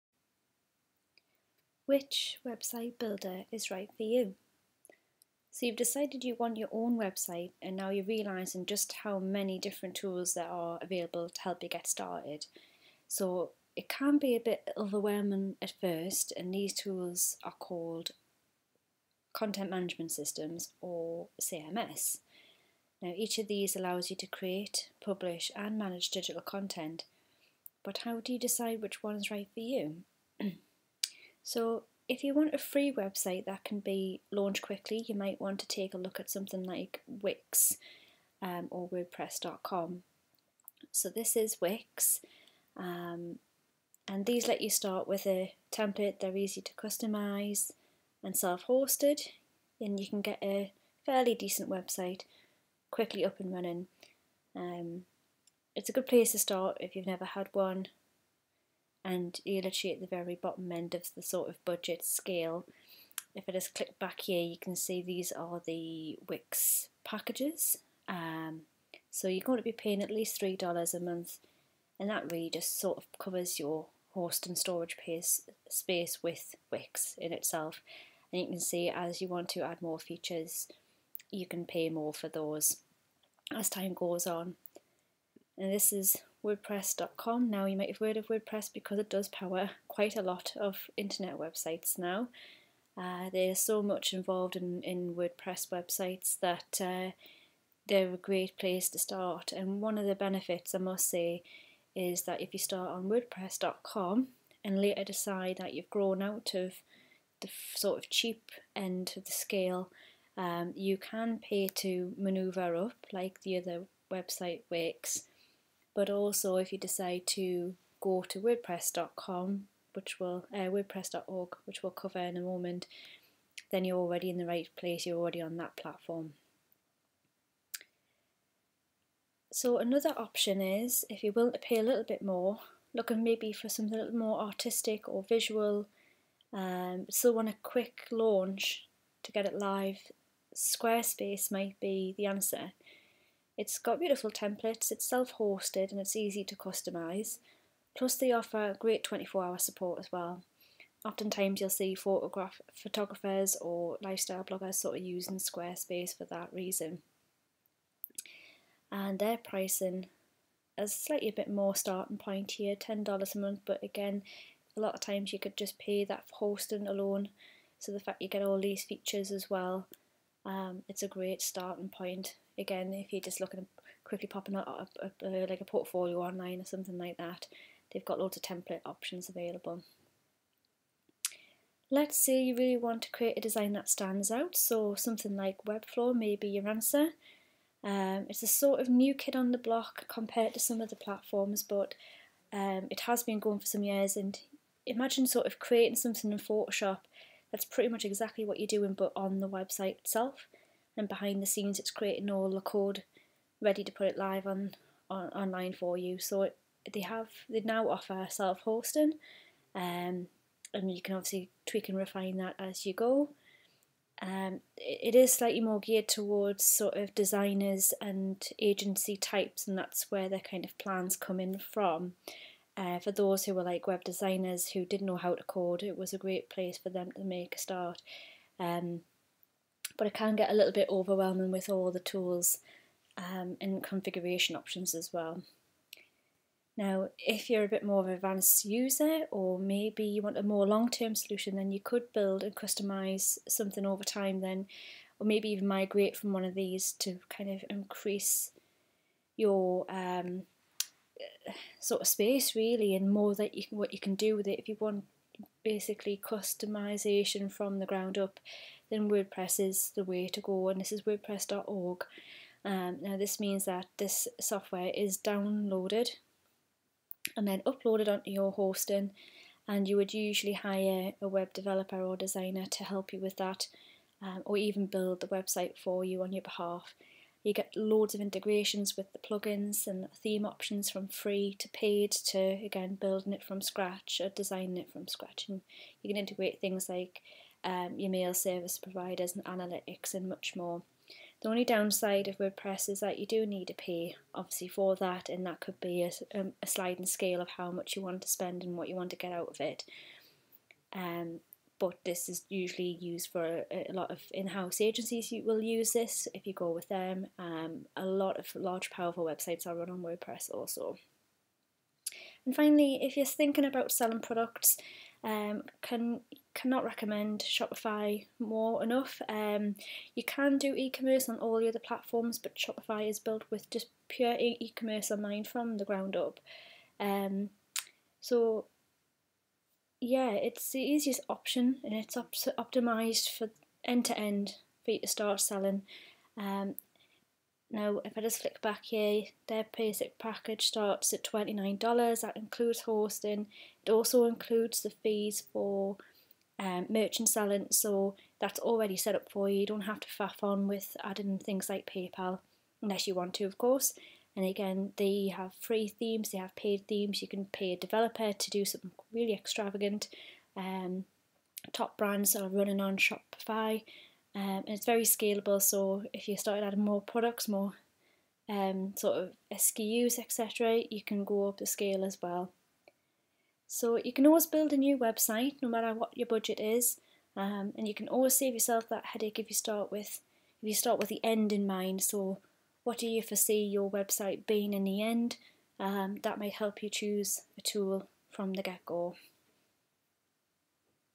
which website builder is right for you
It can be hard to decide on what platform, to build your new website. Choices range in complexity and price, so starting with the right one that fits your needs, will make it easier in the long run. Here we briefly run through the different options available.